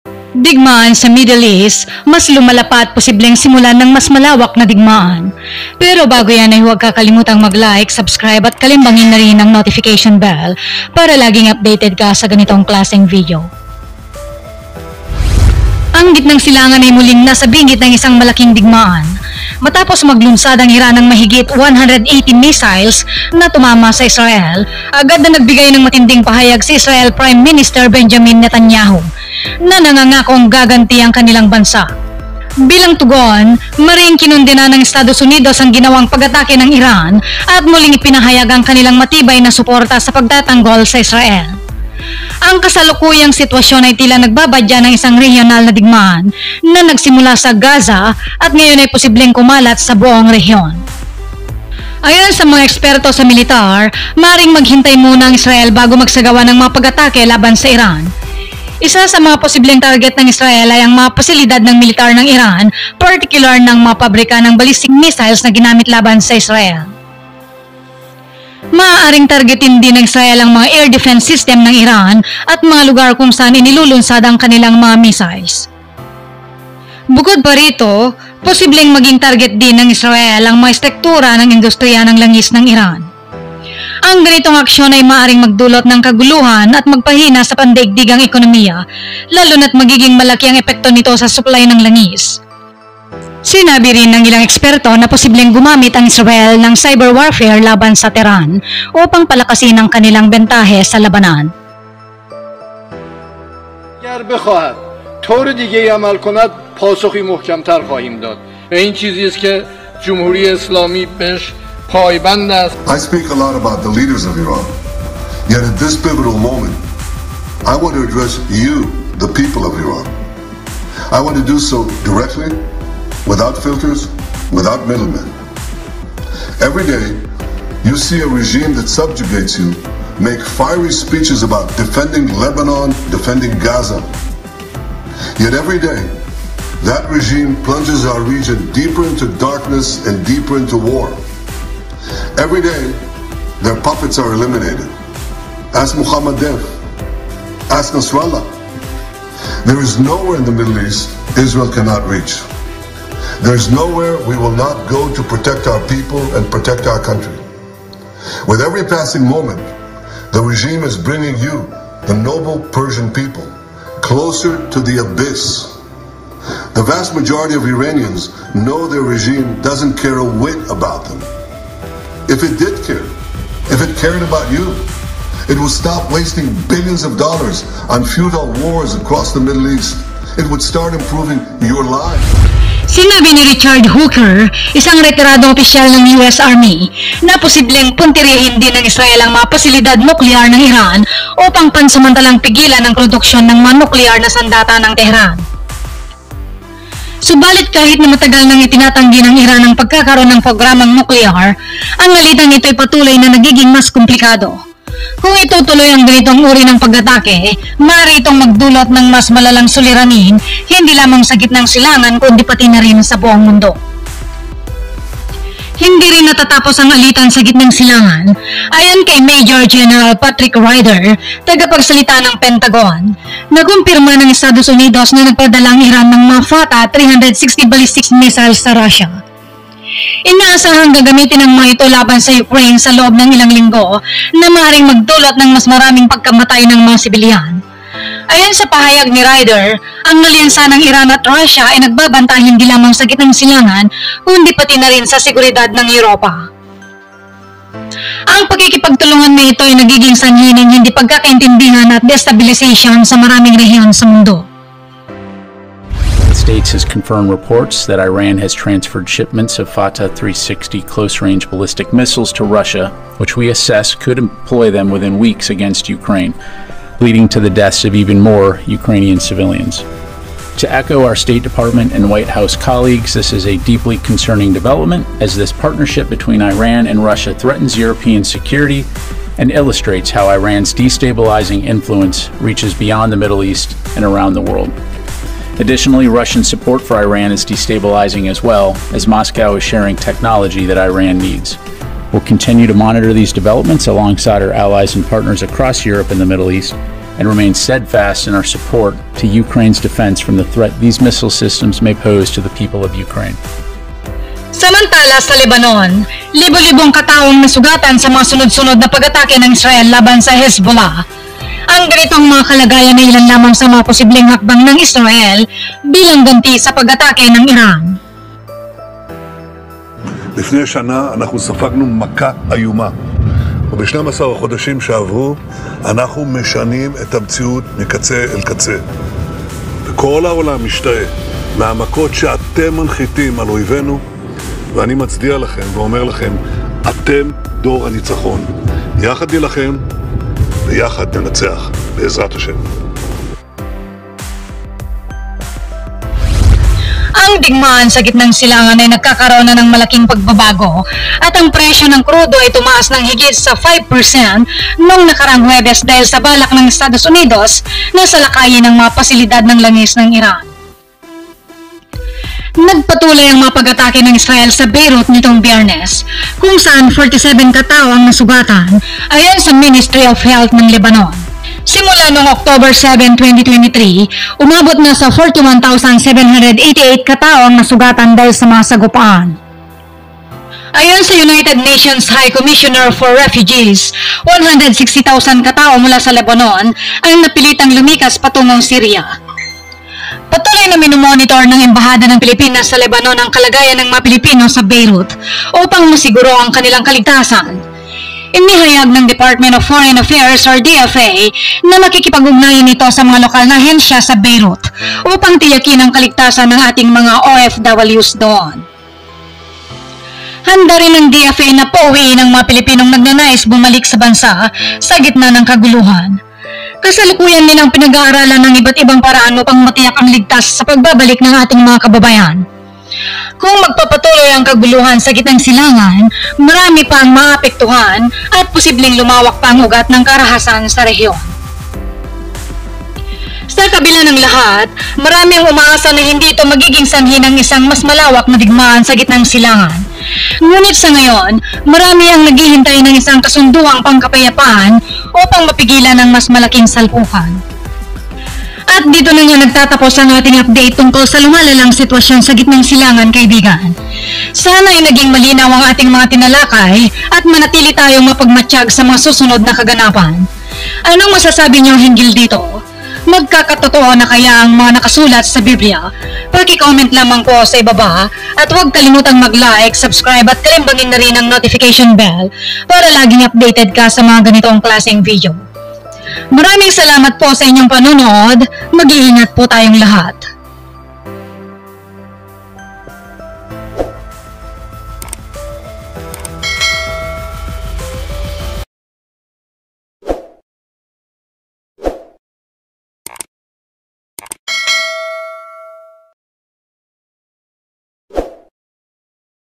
Digmaan sa Middle East mas lumala pa at posibleng simulan ng WW3?
Digmaan sa middle East mas lumala pa at posibleng simulan ng mas malawak na digmaan.

Ang Gitnang Silangan ay muling nasa bingit ng isang malaking digmaan. Matapos maglunsad ang Iran ng mahigit 180 missiles na tumama sa Israel, agad na nagbigay ng matinding pahayag si Israeli Prime Minister Benjamin Netanyahu, na nangakong gaganti ang kanilang bansa. Bilang tugon, mariing kinondena ng Estados Unidos ang ginawang pag-atake ng Iran at muling ipinahayag ang kanilang matibay na suporta sa pagtatanggol sa Israel. Ang kasalukuyang sitwasyon ay tila nagbabadya ng isang rehiyonal na digmaan, na nagsimula sa Gaza at ngayon ay posibleng kumalat sa buong rehiyon.

Ayon sa mga eksperto sa militar, maaaring maghintay muna ang Israel bago magsagawa ng mga pag-atake laban sa Iran. Isa sa mga posibleng target ng Israel ay ang mga pasilidad ng militar ng Iran, partikular na ang mga pabrika ng ballistic missile na ginamit laban sa Israel. Maaaring targetin din ng Israel ang mga air defence system ng Iran at mga lugar kung saan inilulunsad ang kanilang mga missile.
Bukod pa rito, posibleng maging target din ng Israel ang mga estruktura ng industriya ng langis ng Iran. Ang ganitong aksyon ay maaaring magdulot ng kaguluhan at magpahina sa pandaigdigang ekonomiya, lalo na’t magiging malaki ang epekto nito sa suplay ng langis. Sinabirin ng ilang eksperto na posibleng gumamit ang Israel ng cyber warfare laban sa Tehran upang palakasin ang kanilang bentahe sa labanan.